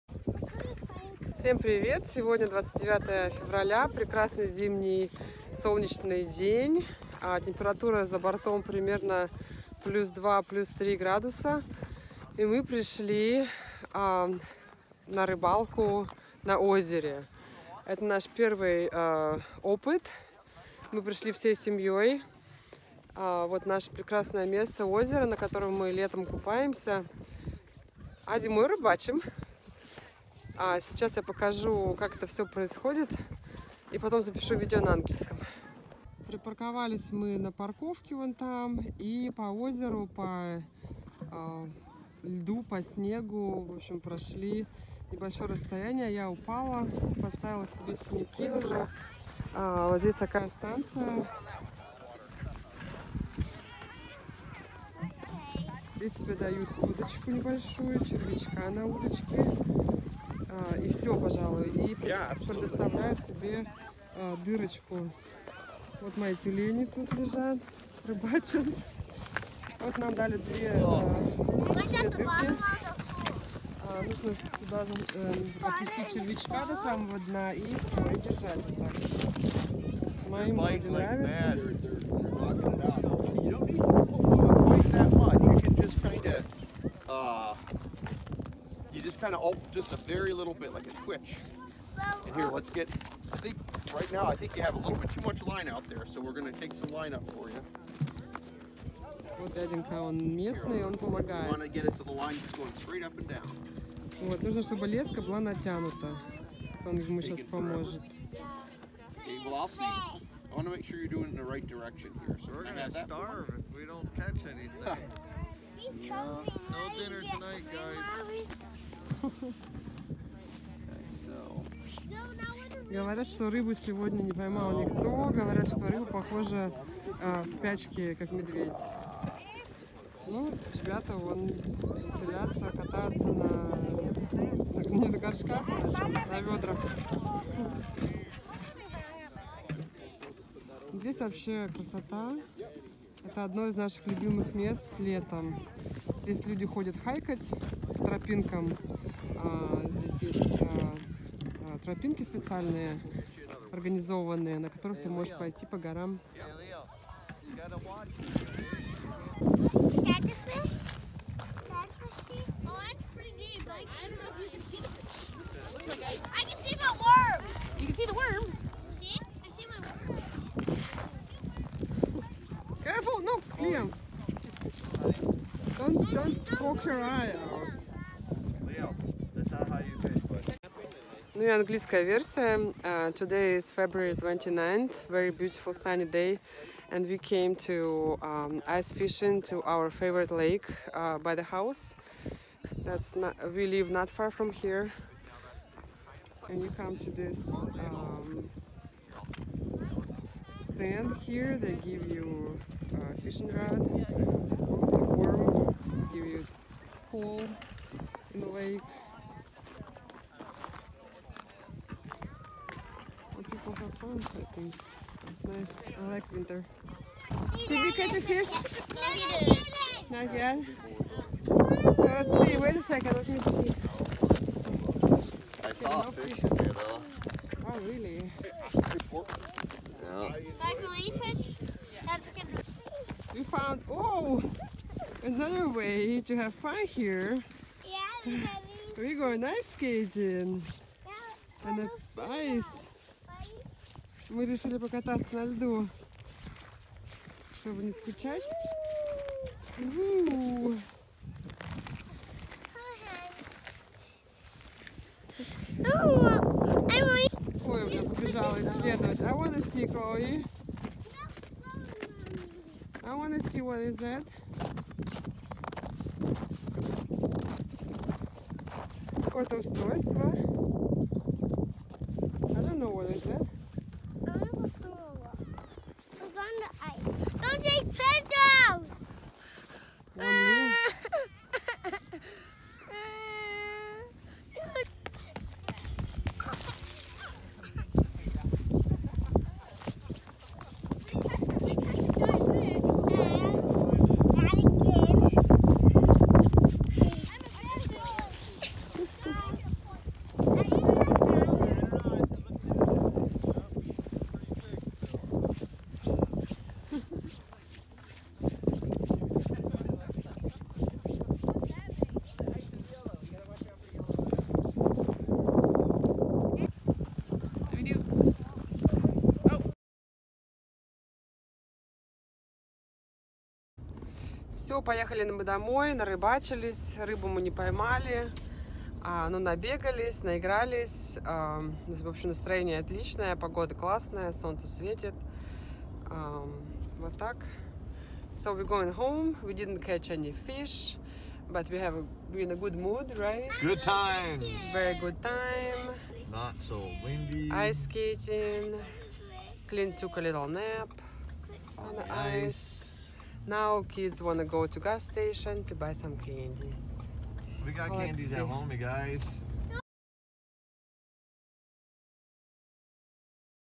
Beautiful Day for Fishing On Ice In America
A day with the family on the Lake a lot of fun but no luck with the fish.